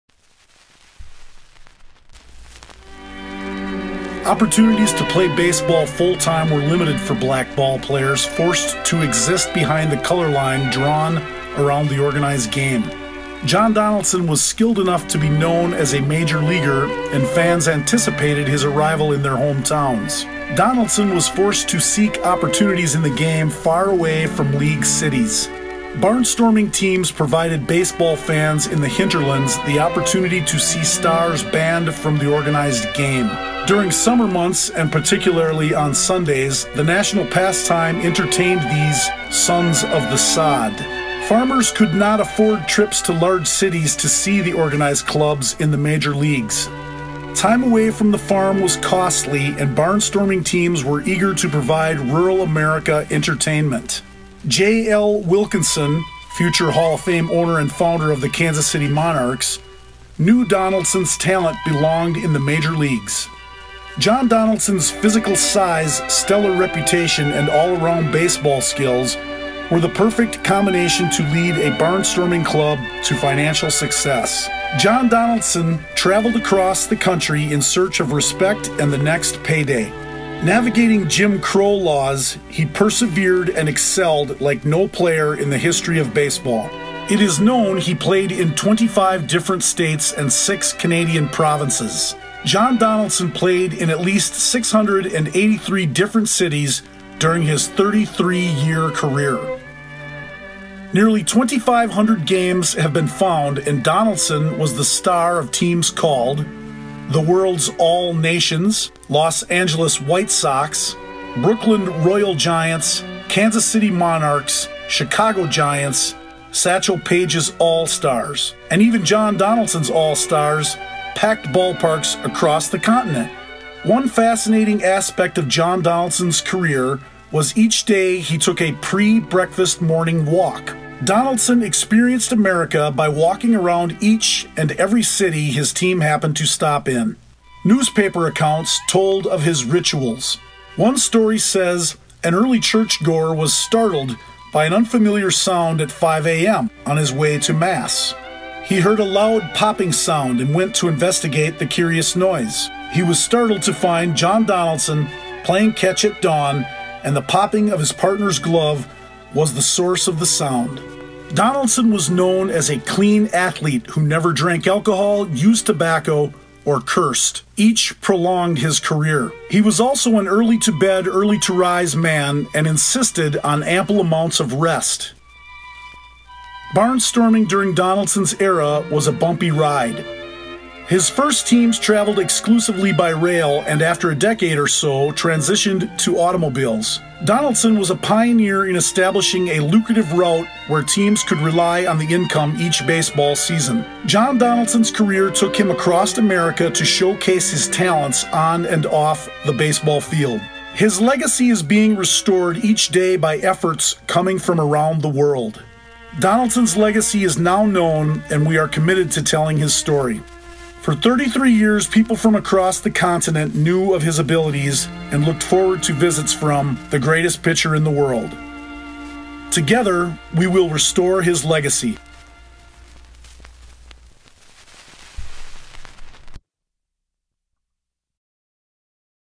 33 Seasons Barnstorming - The Donaldson Network Films
John Wesley Donaldson was known as “The Greatest Colored Pitcher in the World,” when he played baseball from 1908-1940.

Segregation in America forced Donaldson to play anywhere but a major league field.

The Donaldson Network is working to change what history remembers of him.

This film tells about the 33-seasons John Donaldson played baseball on the outside looking in.

Organized baseball kept Donaldson and others who looked like him off of major league fields with their “Gentleman’s Agreement.” This meant a life traveling through Jim Crow America surviving playing the National game in obscurity.

Donaldson’s career is now known and we are actively restoring his legacy.

More than a baseball story, more than a Negro League legend, John Donaldson performed in nearly 700 towns and cities across the continent.

John Donaldson played for some of the greatest franchises in the history of Negro Leagues baseball. Starting with J.L. Wilkinson’s World All Nation’s, the Indianapolis A.B.C.’s, Brooklyn Royal Giants, Los Angeles White Sox, Chicago American Giants, Kansas City Monarchs, Chicago Giants, Satchel Paige’s All Stars and even John Donaldson’s All Stars.

The Donaldson Network was formed in 2005 to discover every game John Donaldson played in.

More information can be found at:  johndonaldson.bravehost.com.

Our effort has been decorated with accolades including the SABR- Negro Leagues Committee Tweed Webb Lifetime Achievement Award and called by The Center for Negro Leagues Baseball Research, “The most extensive research project that has ever been undertaken related to black baseball.”

Our short film series will define John Donaldson’s career.

Together we will restore his legacy!